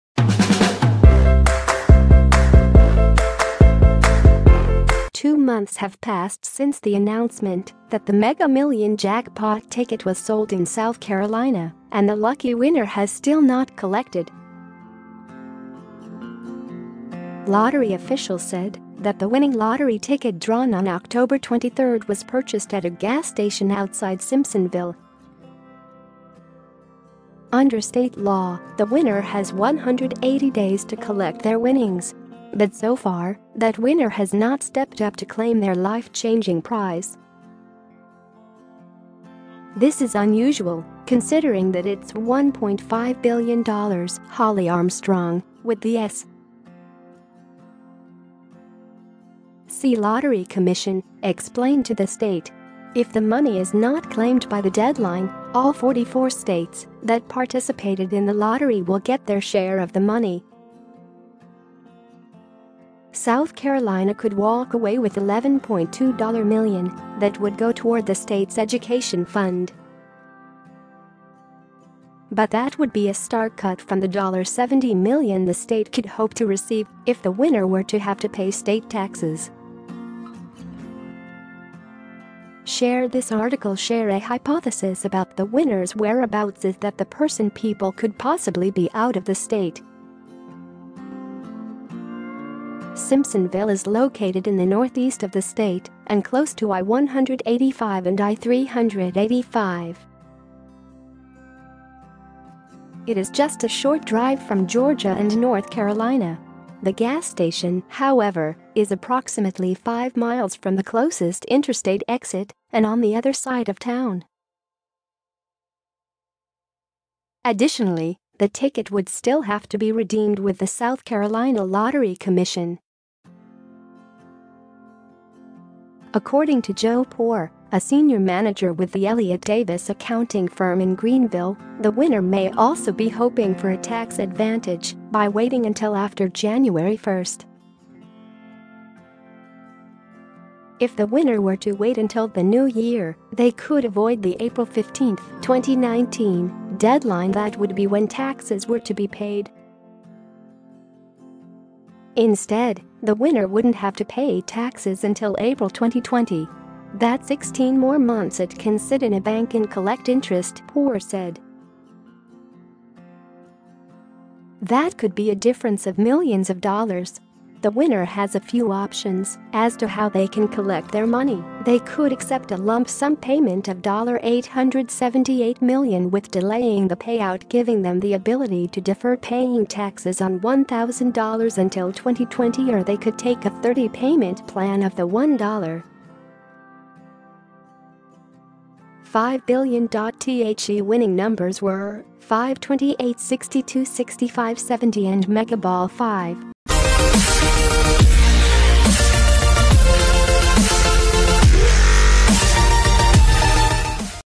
Winner of $1.5B Mega Million jackpot STILL hasn't collected winnings - Daily News
Winner,1,5B,Mega,Million,jackpot,fetched,winnings,South,Carolina Two months have passed since the announcement that the Mega Million jackpot ticket was sold in South Carolina, and the lucky winner has still not collected.Lottery officials said that the winning lottery ticket - drawn on October 23 - was purchased at a gas station outside Simpsonville. Under state law, the winner has 180 days to collect their winnings. But so far, that winner has not stepped up to claim their life changing prize. 'This is unusual, considering that it's $1.5 billion,' Holli Armstrong, with the S.C. Lottery Commission, explained to The State.    If the money is not claimed by the deadline, all 44 states that participated in the lottery will get their share of the money. South Carolina could walk away with $11.2million that would go toward the state's education fund. But that would be a stark cut from the $70million the state could hope to receive if the winner were to have to pay state taxes.    Share this article   Share   A hypothesis about the winner's whereabouts is that the person/people could possibly be out of the state. Simpsonville is located in the northeast of the state and close to I-185 and I-385. It is just a short drive from Georgia and North Carolina.   The gas station, however, is approximately five miles from the closest interstate exit and on the other side of town. Additionally, the ticket would still have to be redeemed with the South Carolina Lottery Commission. According to Joe Poore, a senior manager with the Elliot Davis accounting firm in Greenville, the winner may also be hoping for a tax advantage by waiting until after January 1st.   If the winner were to wait until the new year, they could avoid the April 15, 2019, deadline that would be when taxes were to be paid. Instead, the winner wouldn't have to pay taxes until April 2020. 'That's 16 more months it can sit in a bank and collect interest,' Poore said. 'That could be a difference of millions of dollars.' The winner has a few options as to how they can collect their money: they could accept a lump-sum payment of $878million with delaying the payout giving them the ability to defer paying taxes on $1000 until 2020 or they could take a 30 payment plan of the $1.5billion.The winning numbers were: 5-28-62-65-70 and Mega Ball 5.